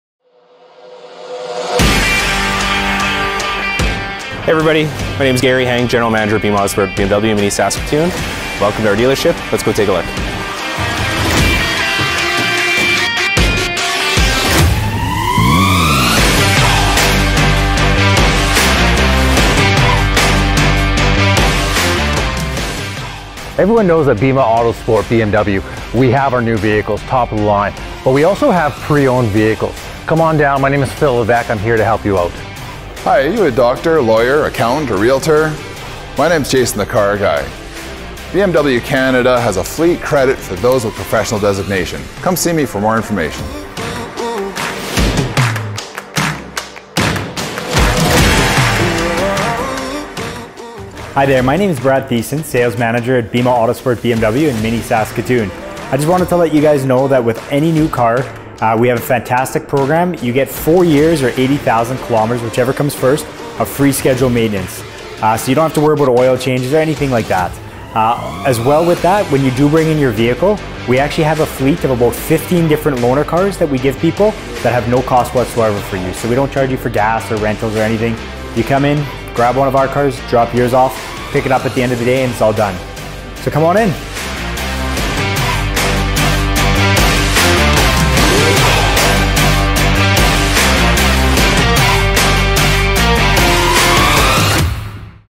Bema Autosport BMW - Dealership Introduction
Just a small glimpse into our dealership and what we have to offer.